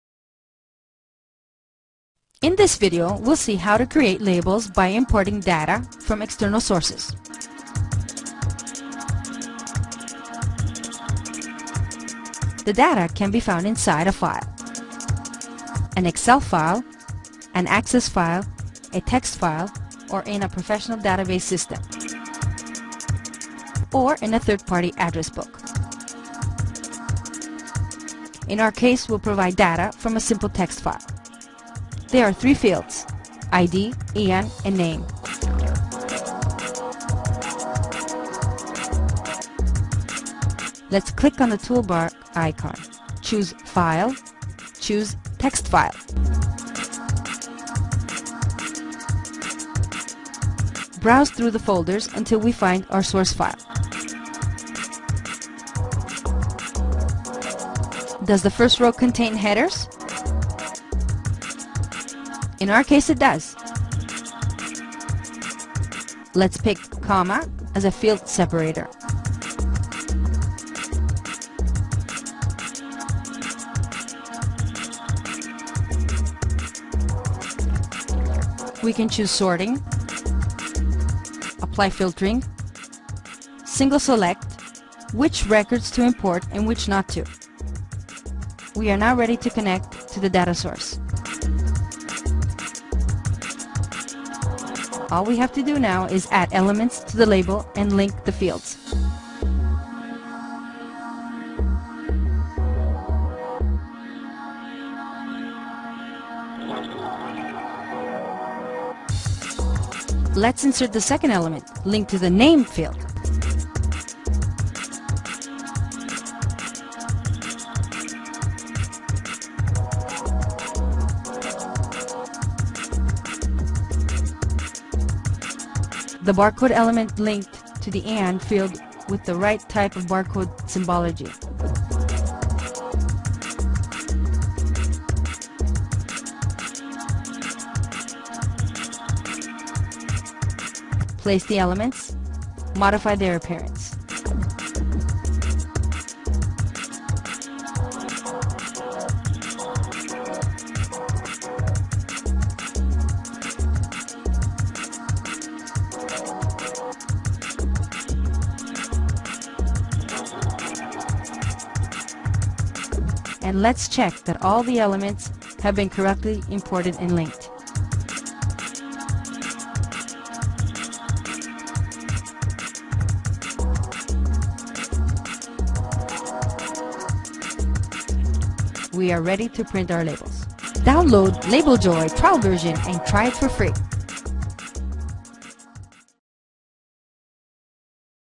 Labeljoy loads external data source and print label, barcode, qrcode
How to create labels importing data from external data sources.
Data can be imported from Access ©, Excel ©, Sql Server ©, Oracle ©, MySql ©, Lotus notes ©, Dbase, Txt, Csv, Windows address book ©, Outlook ©, Sendblaster ©,  wk1-2-3.